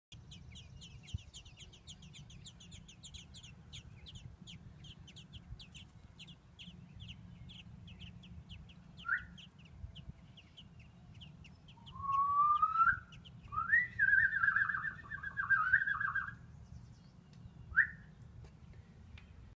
Little Birdies in Spring
Little Robins born in Spring on my front porch